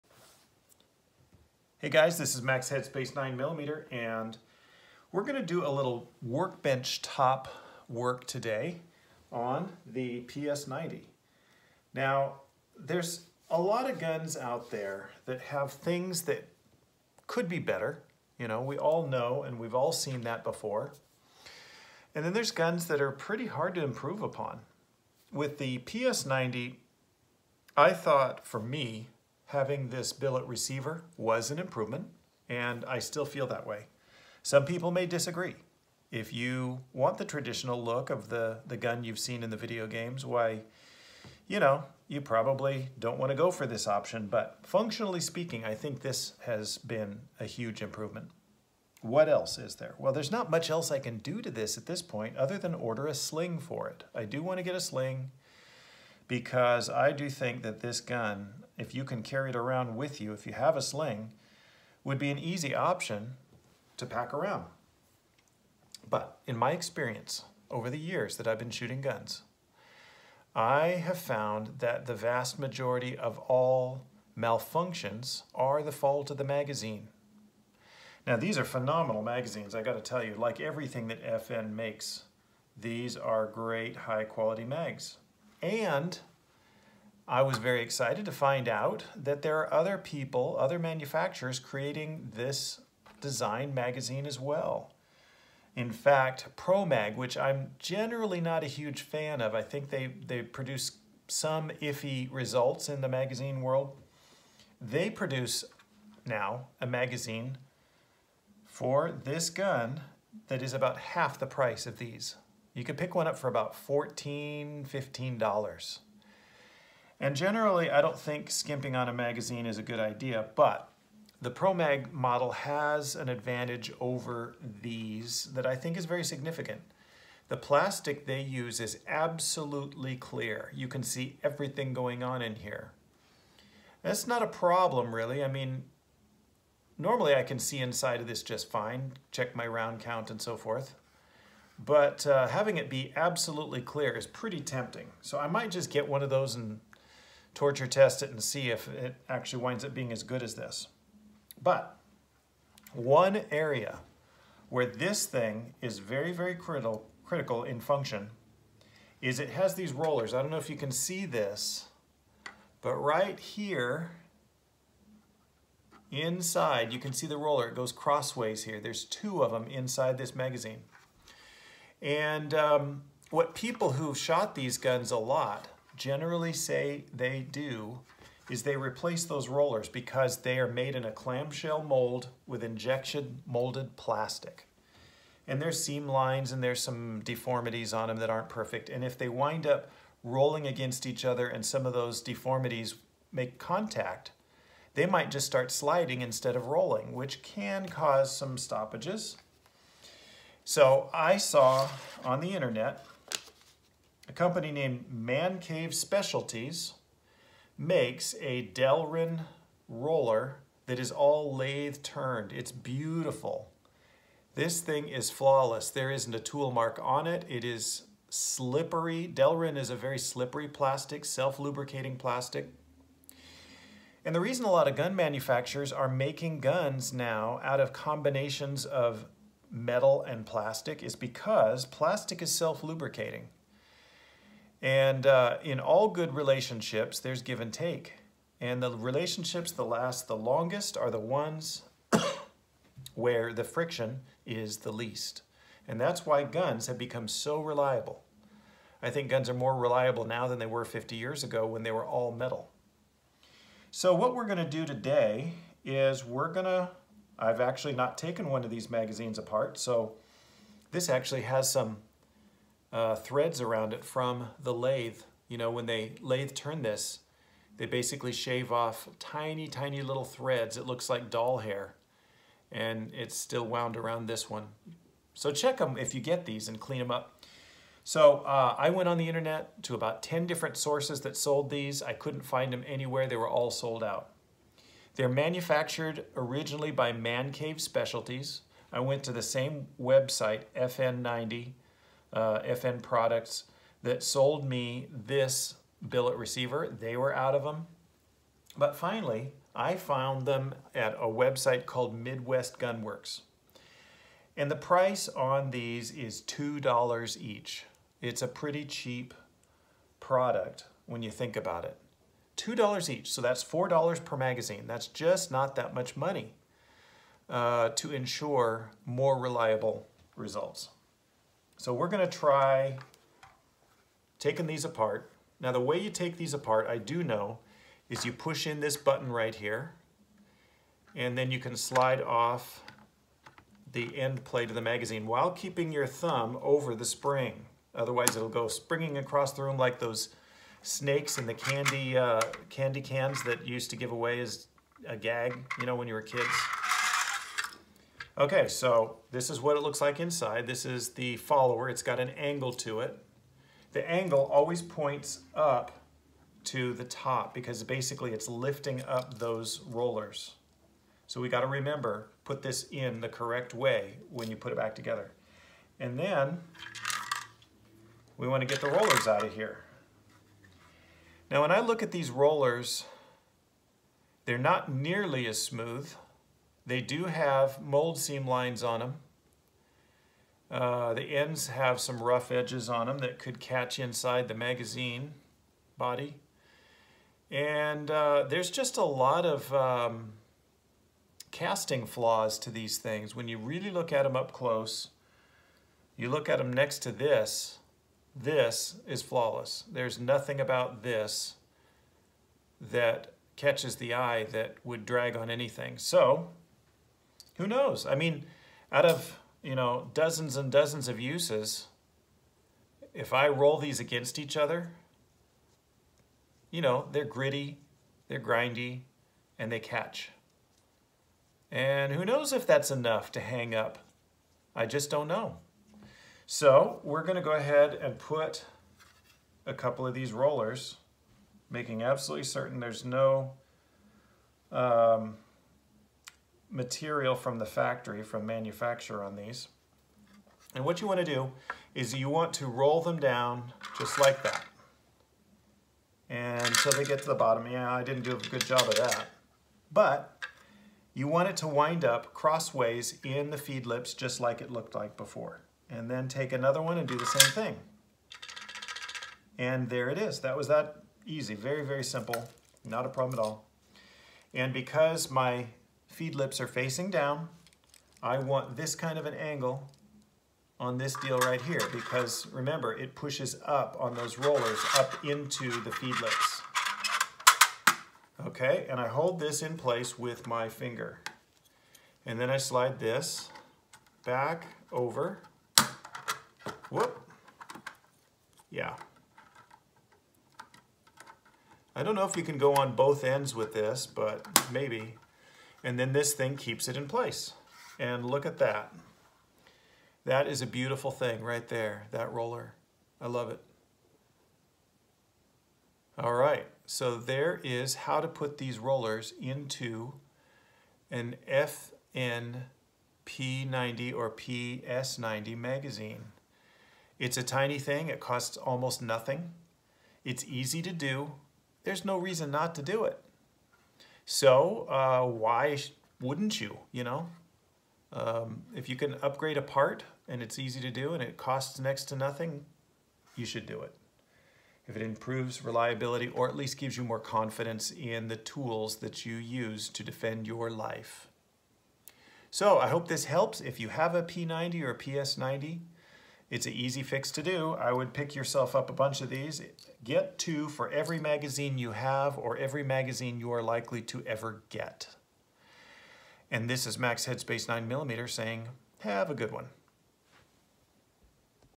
PS90 Mag Mod
A cheap, fast and easy way to make one of the best little guns in the world even better!